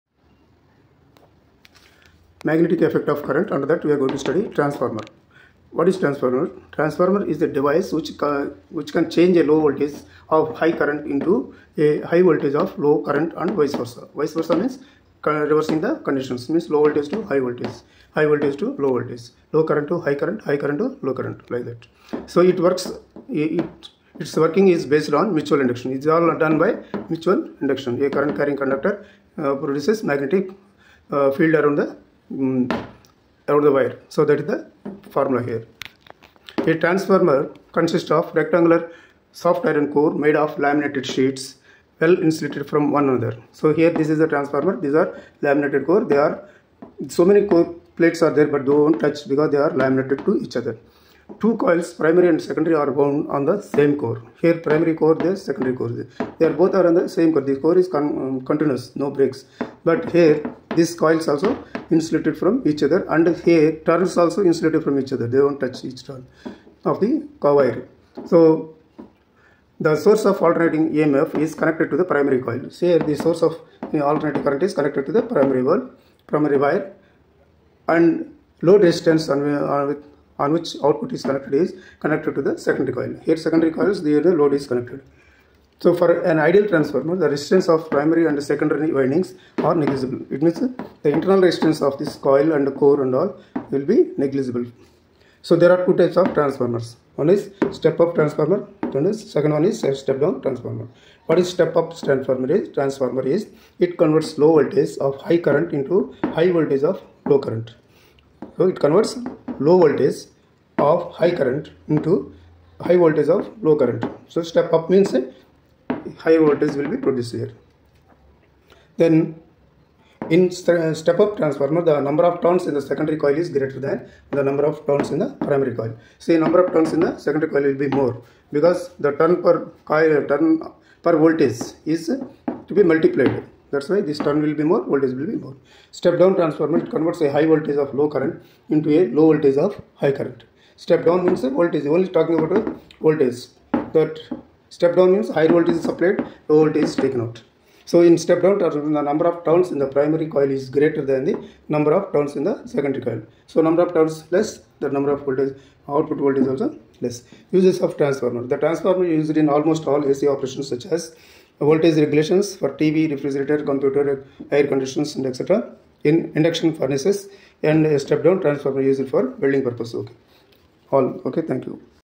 Transformer: Construction, types of transformers and thier uses
your queries.
principle of transformers
# construction of transformers
step up transformer
stepdown transformer
uses of transformers
feed